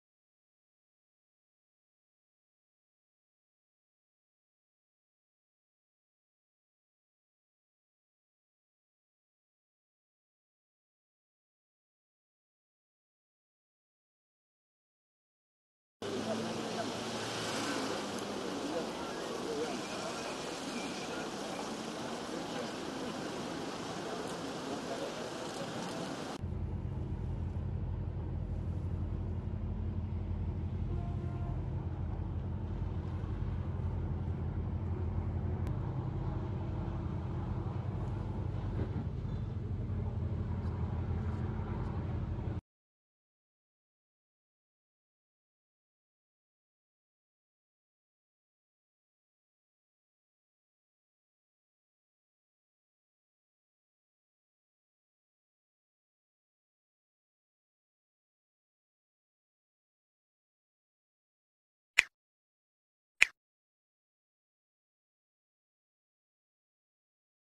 Watch: Turkey's historic Wooden Vanikoy Mosque catches fire
The historic wooden Vanikoy Mosque, built in the 17th century, has caught on fire in Istanbul.